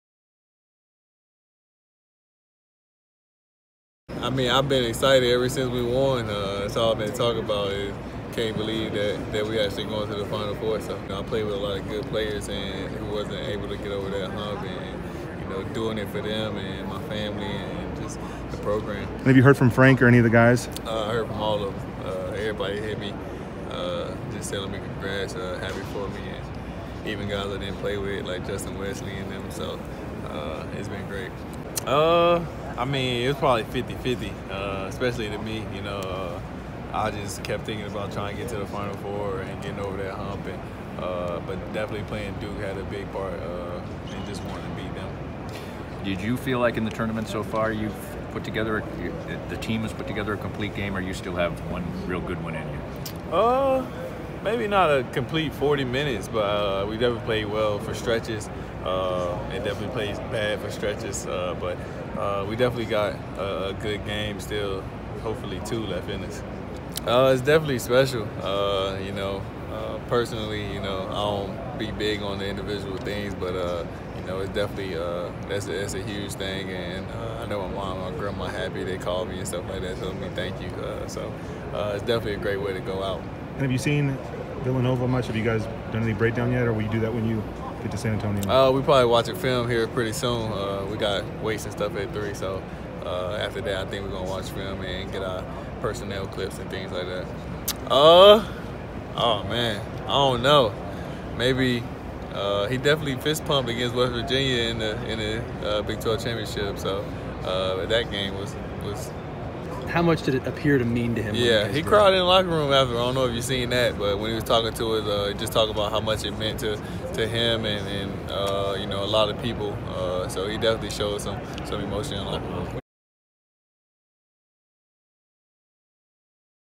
Devonte' Graham talks Final Four excitement, being named All-American
Since the clock reached zero on Sunday and Lagerald Vick threw the ball up into the air with two hands, Devonte' Graham has been ready for what comes next.

"I"ve been excited ever since we've won," Graham said. "I've played with a lot of good players who wasn't able to get over that hump."

Some of those players, including Frank Mason, have reached out to the team to express their congratulations. Graham, who was named a first team All-American by the Associated Press on Tuesday, said the outpour of well-wishes has been widespread.

"I've heard from all of them," Graham said. "Even guys I didn't play with."